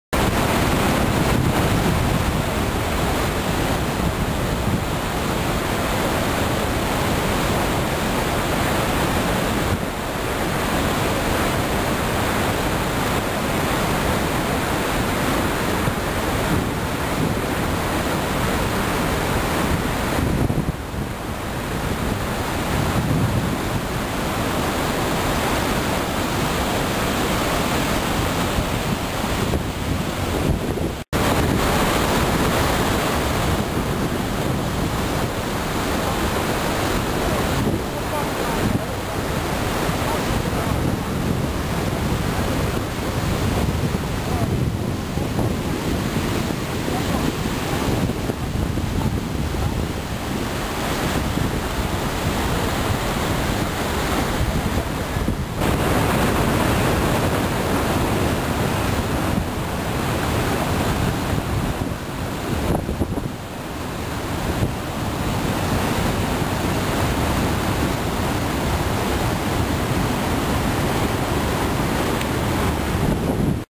Iguazu-Falls
The falls are the worlds second biggest, after Victoria Falls (and peak water flow is almost 2½ x that of Niagara), and are particularly interesting because the waterfall system, up to 80m high, consists of about 270 cataracts, some quite small, along 2.7km of the Iguazu river. There are many islands that split this length. The largest fall, the Devils Throat, is U-shaped, 150m wide and 700m long, and it is at the border of Brazil and Argentina.